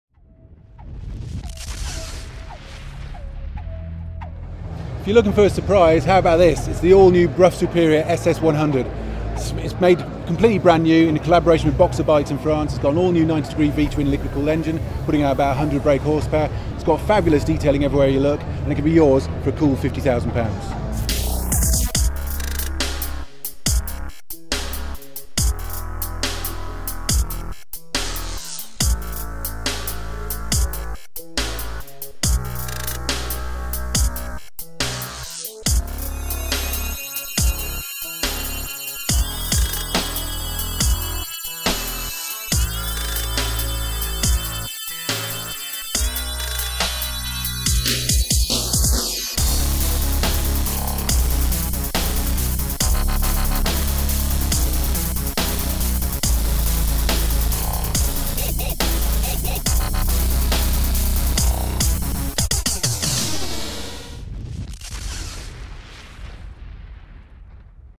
All new Brough Superior S.S.100 | First look | Motorcyclenews.com
For €50,000 (not £50k we said in the video) you can me the proud owner of an all new Brough Superior S.S.100.